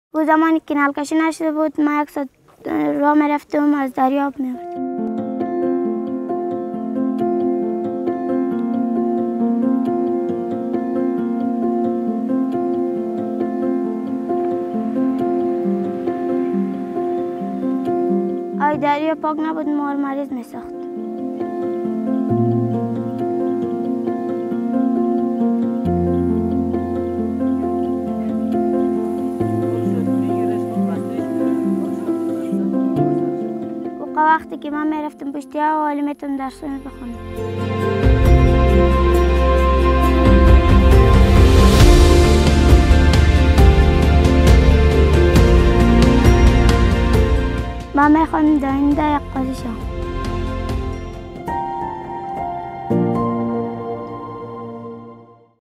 Clean water made possible in remote areas of Afghanistan
Thanks to the support from Governments of Australia, Canada, Finland, Japan, Korea, and Sweden, with partners, UNICEF developed piped water network systems in remote areas of Afghanistan.   Click the video to see the story of Massoda, 11 from Bamyan a central province of Afghanistan, and how access to clean water changed her life.